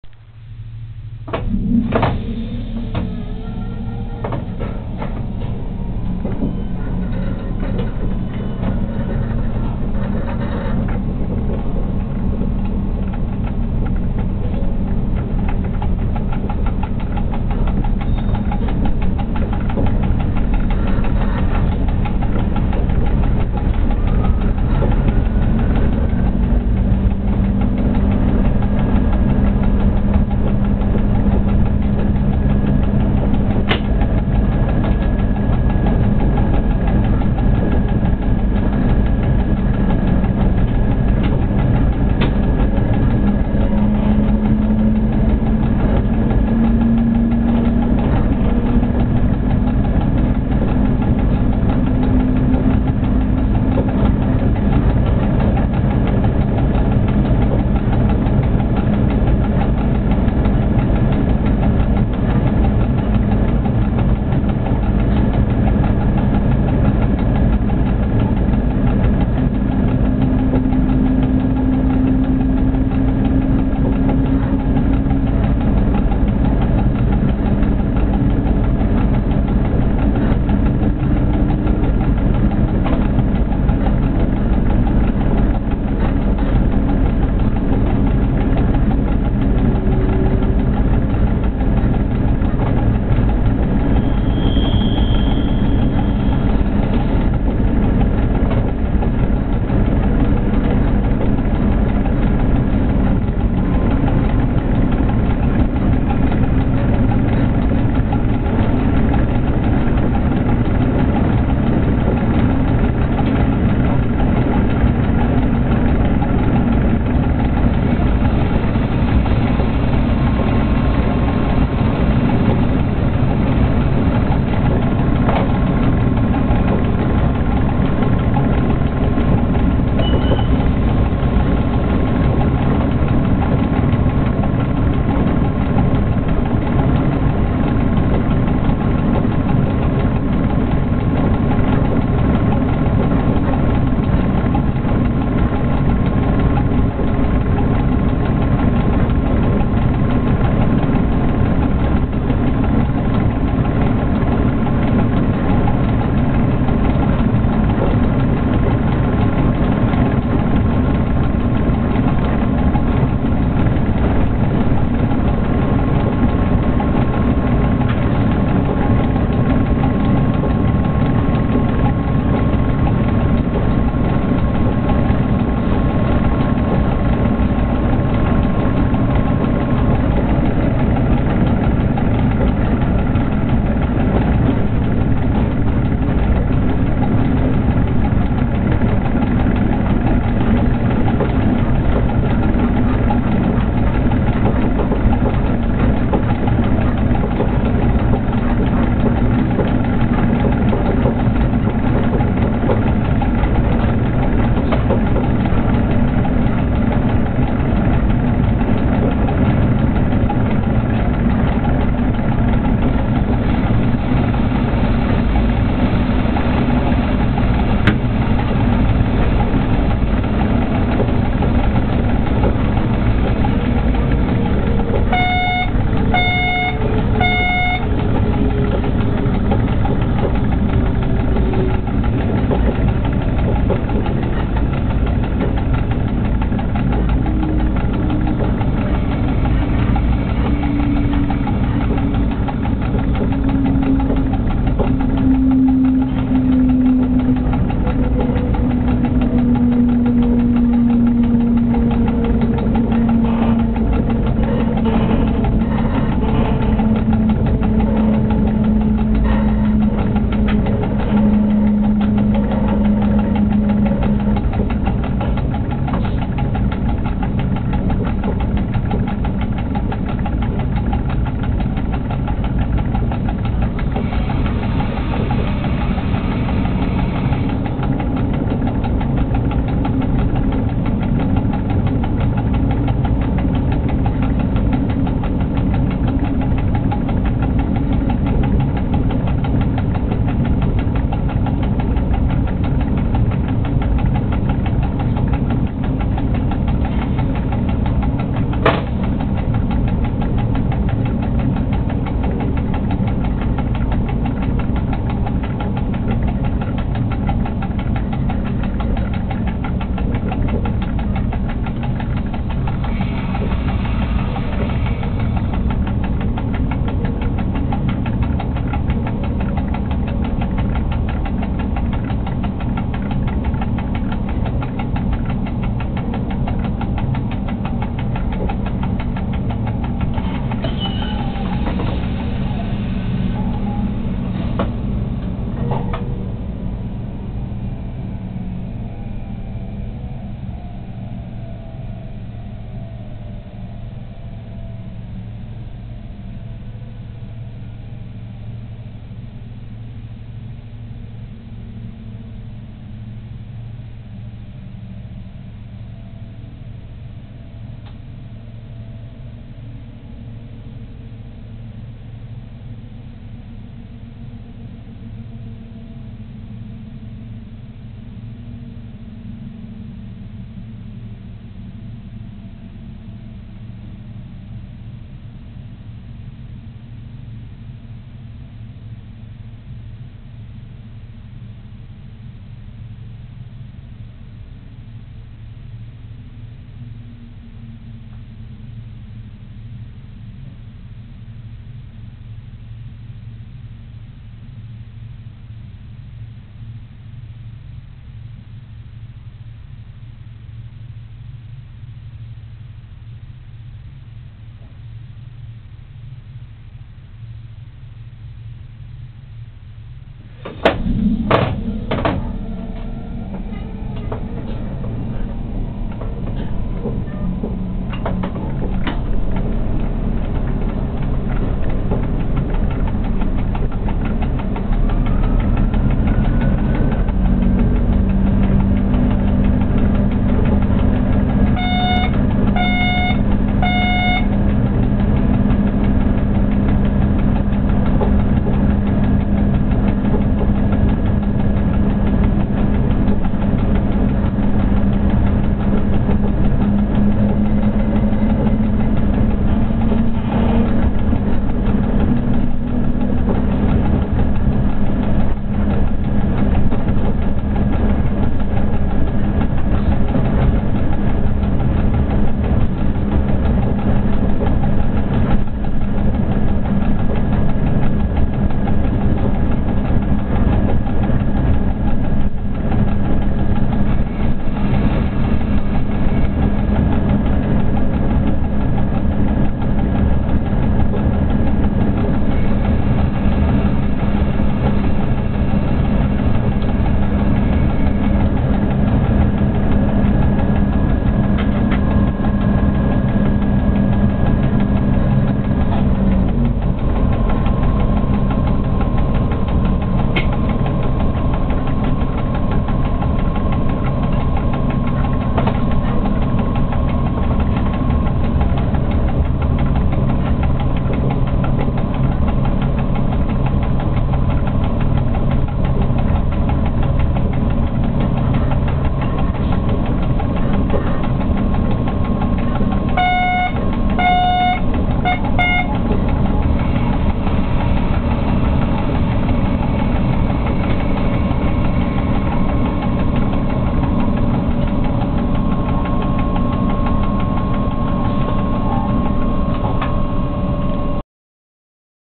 SBB Cargo Lokführerfahrt Rheintal 1
Fahrt von Heerbrugg mit dem morgendlichen 64316 weiter nach St.Margrethen im Schnee.